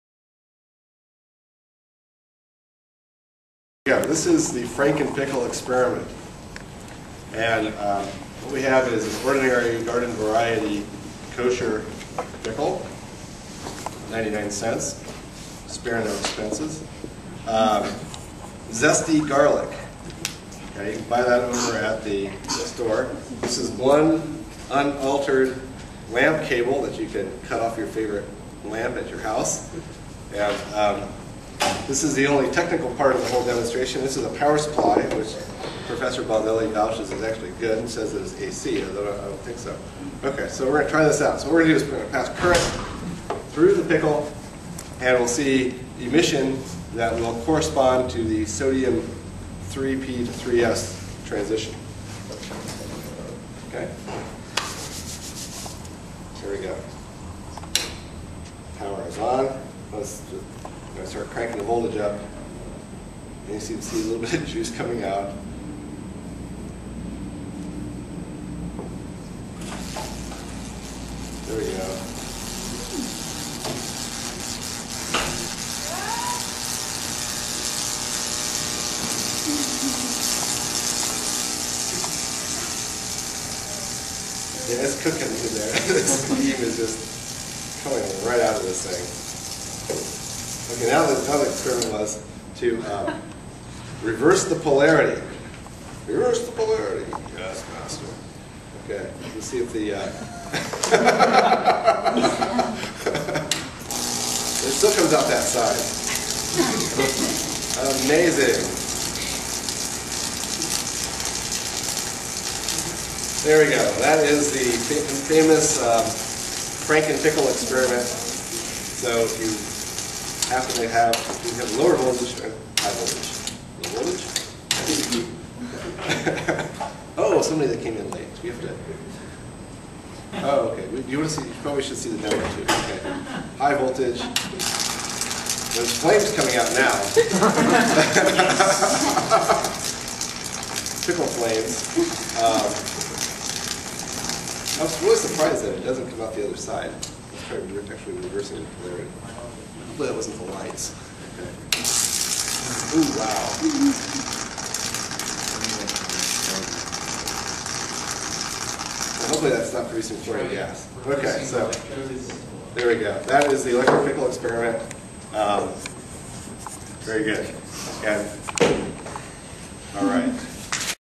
Dr. Bittner and The Electric Pickle 2
Dr. Bittner does the electric pickle experiment for a second time.